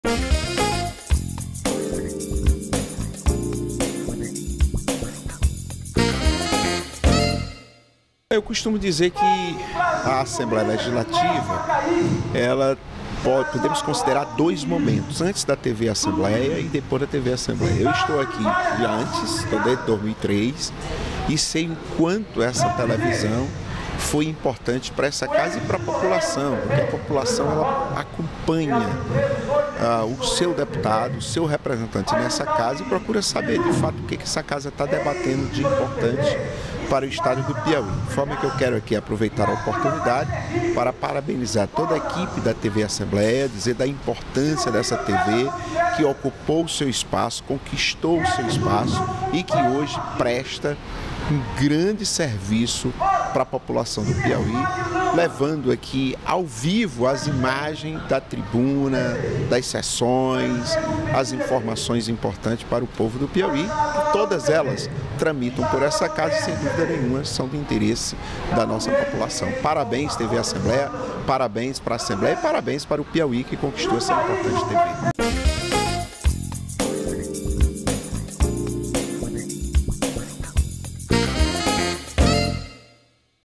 Deputado João de Deus exalta o trabalho prestado durante os 9 anos de TV Alepi
Deputado estadual João de Deus (PT), parabeniza TV Assembleia e fala sobre o grande serviço que a emissora presta a população do Piauí.